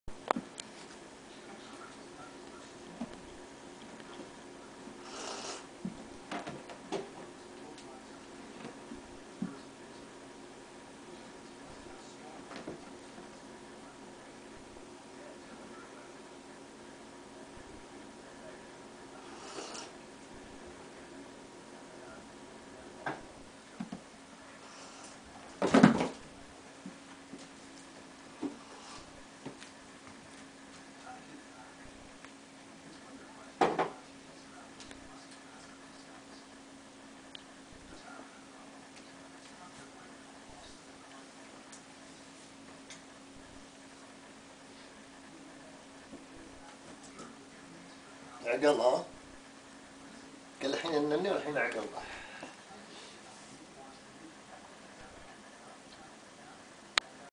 Laith, our beloved kitten; bought in Calgary and died in Saihat, most probably due to a heat stroke.